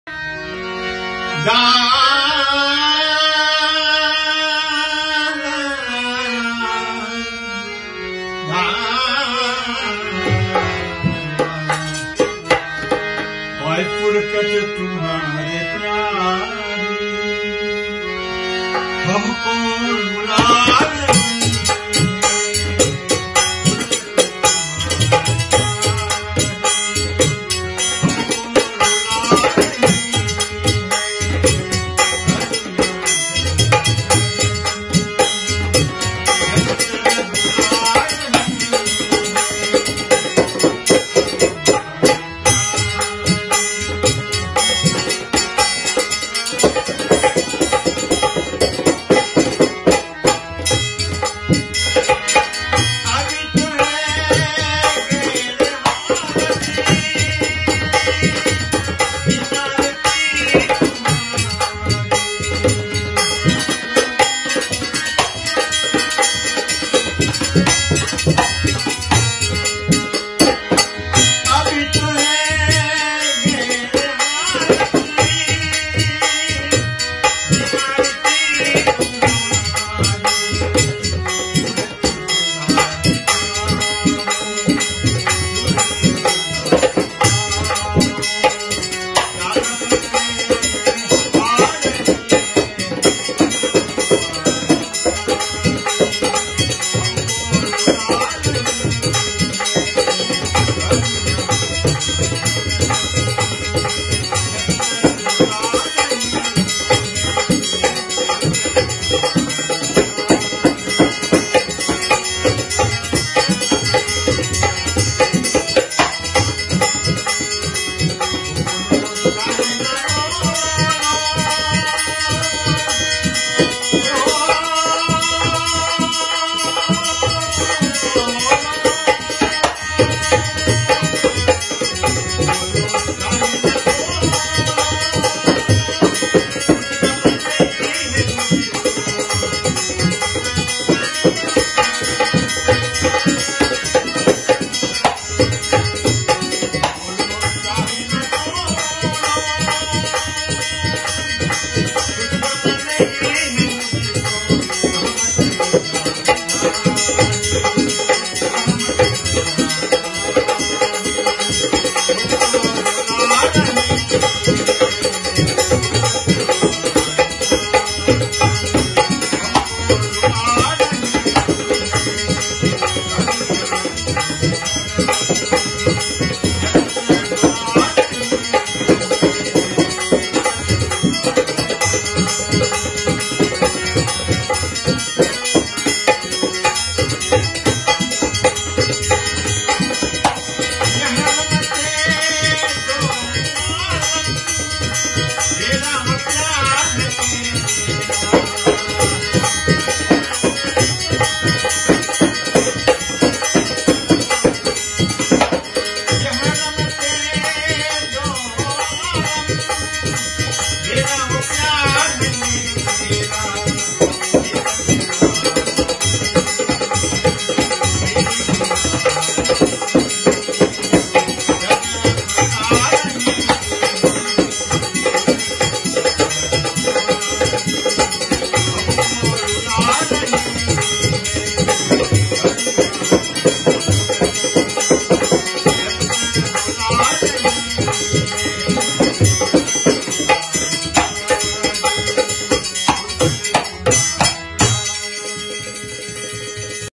Devanand Sookram Ghazal Trinidad Classical
Devanand Sookram - Singer
Kavi Gobin - Drummer
Damian Sookram - Dantal